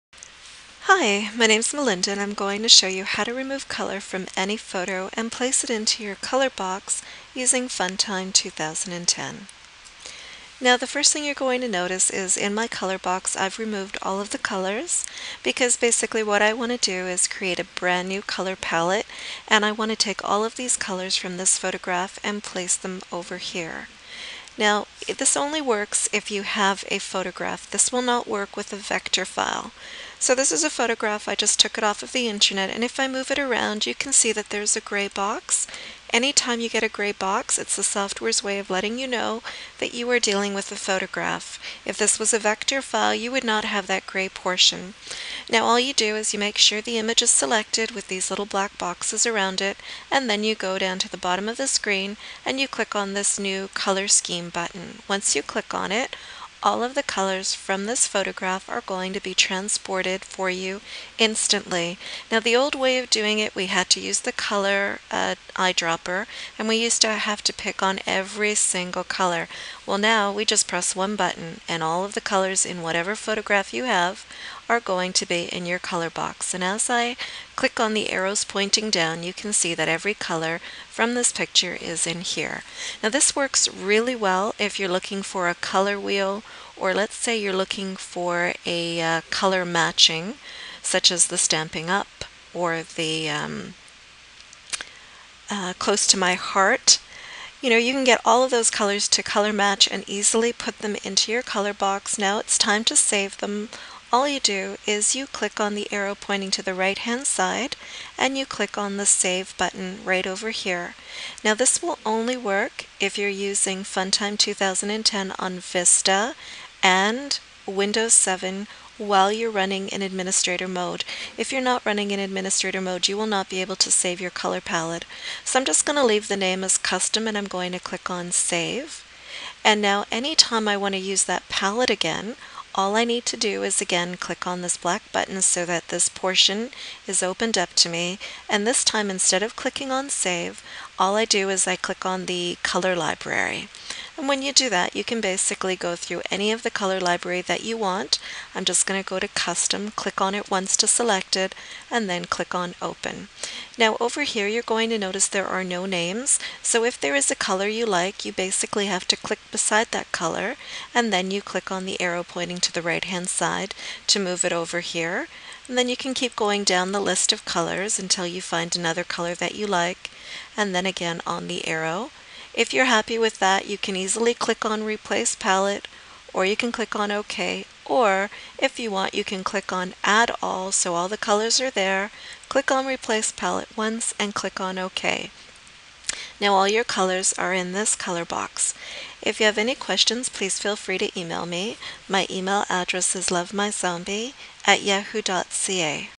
Funtime 2010. Remove Color from Any Photo
Funtime Scrapbooking 2010 (Die Cutting Software). How to Remove Color from any Photo.